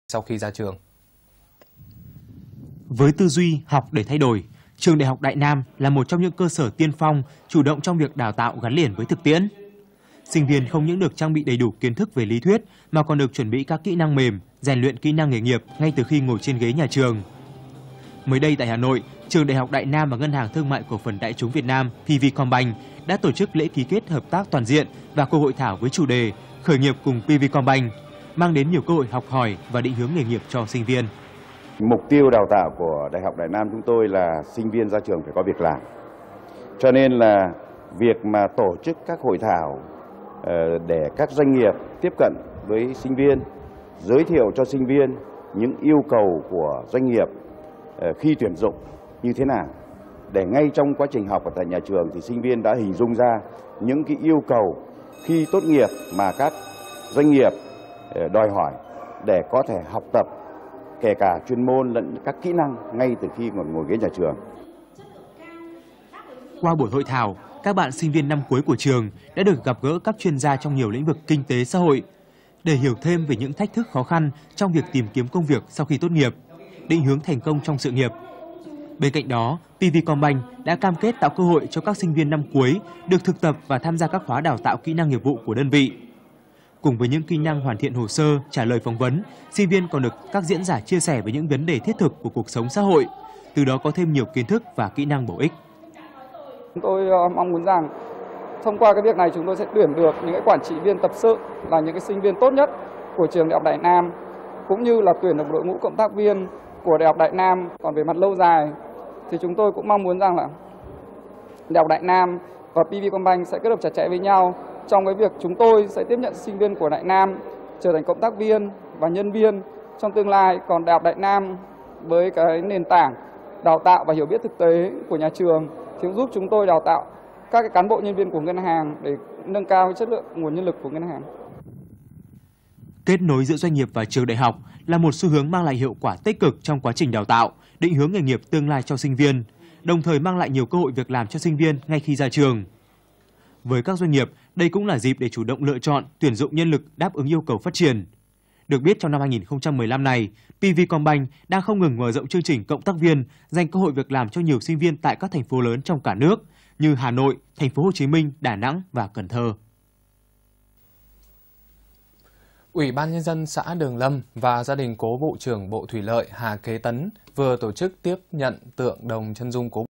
Đại học Đại Nam ký kết hợp tác toàn diện cùng ngân hàng thương mại cổ phần Đại chúng PV combank
Khởi nghiệp cùng PV Combank (ngày 29/1/2015)